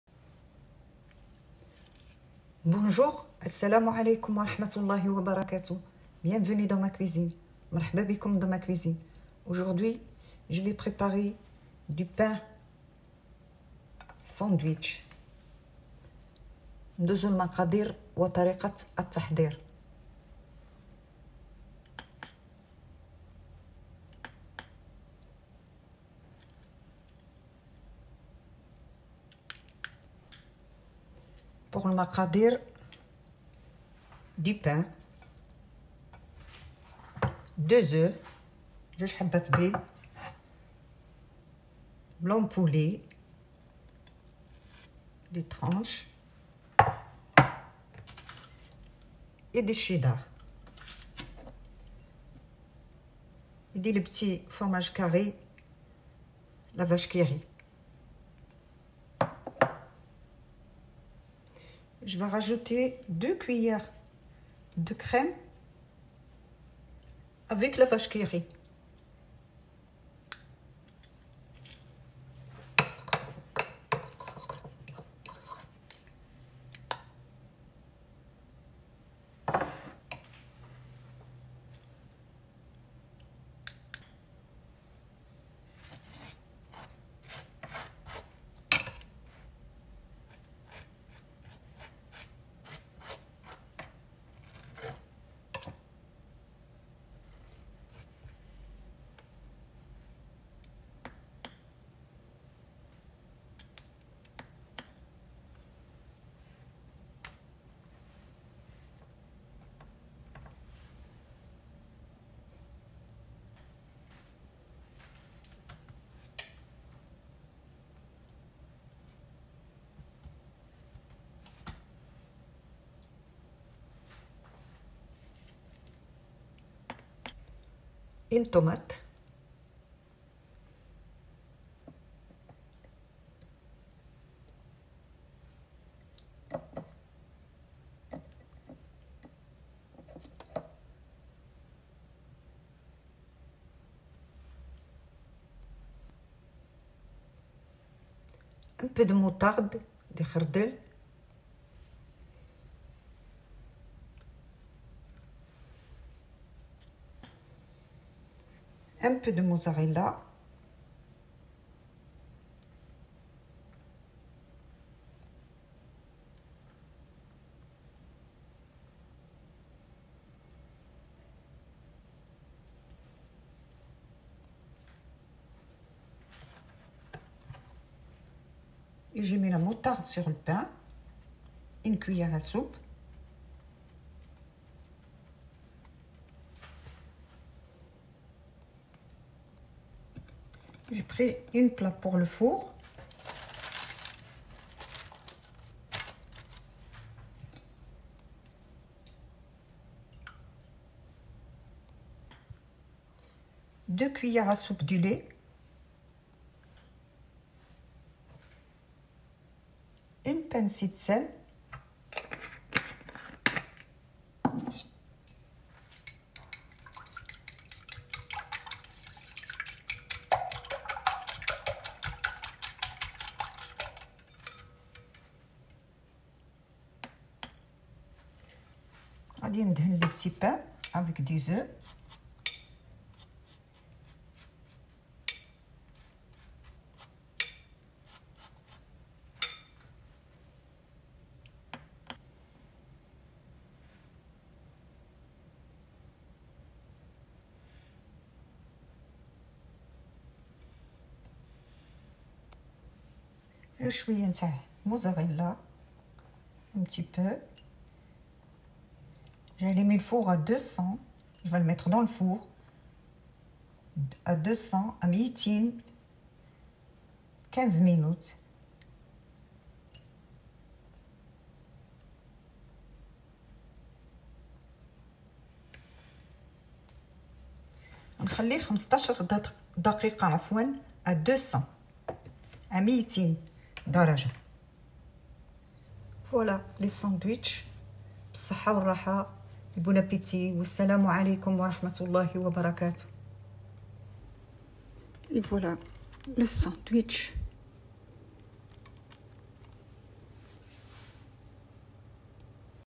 Sandwich au four شطيرة مخبوزة
Ingrédients :

2 tranches de pain
2 œufs
Du cheddar
Du blanc de poulet
6 vaches kiri
1 cas de crème fraîche
1 tomate
Mozzarella

Bonne appétit

مكونات :

شريحتان من الخبز
2 بيض
جبنة الشيدر
صدر دجاج
6 أبقار كيري
1 علبة قشطة طازجة
1 طماطم
موزاريلا

شهية طيبة